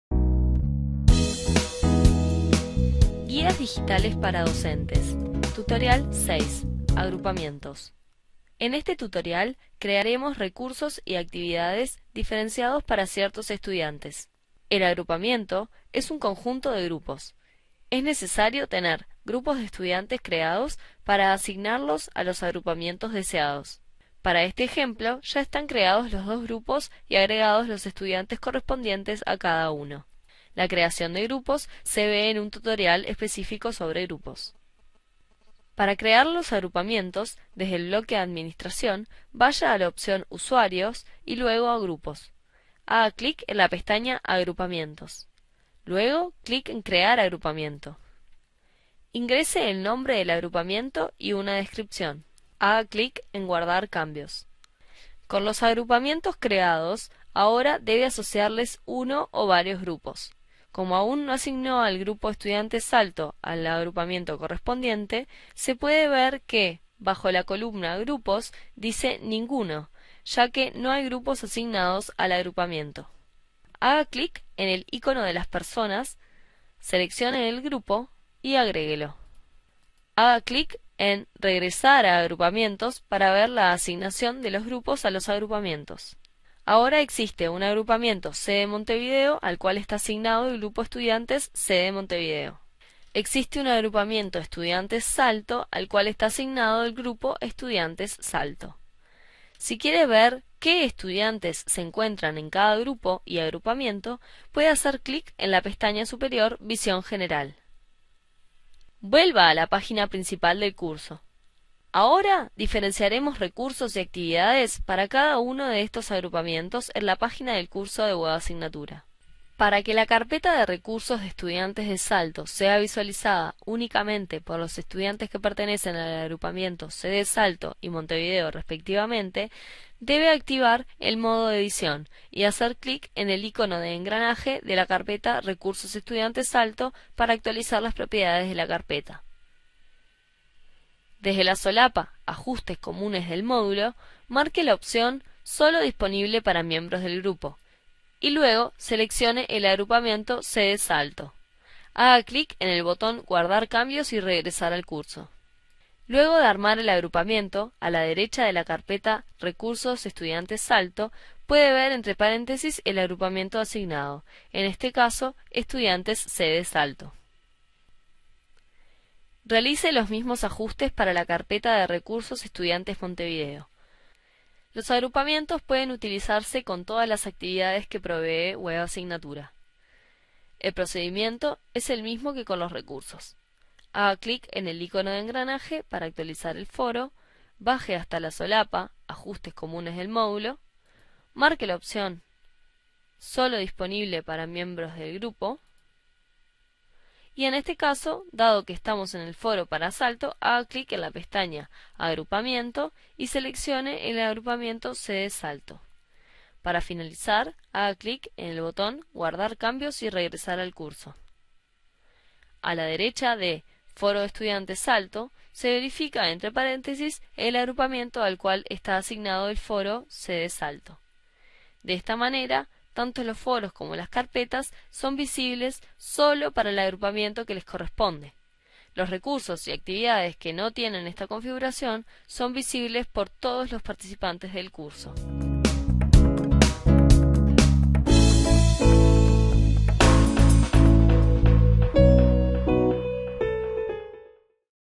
Webasignatura – Agrupamientos (Tutorial 6 para docentes)
Este tutorial es un recurso educativo abierto sobre las principales funciones del Editor de texto en la plataforma educativa virtual Moodle de la Universidad Católica del Uruguay: Webasignatura.